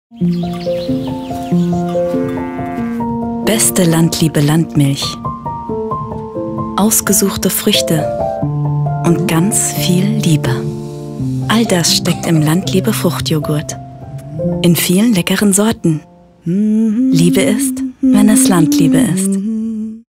Landliebe - Fruchtjoghurt
TV Spot for Landliebe as Digital Intermediate Technician and filmed on location in Cape Town, South Africa